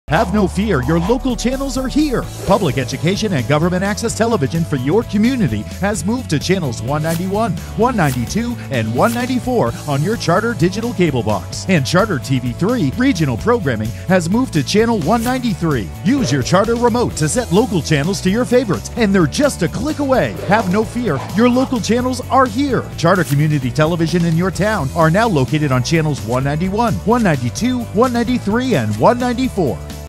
New Local Cable Channels
SCA channel 11 is now channel 191
SCA channel 12 is now channel 192
SCA channel 13 is now channel 194
Charter TV3 is now channel 193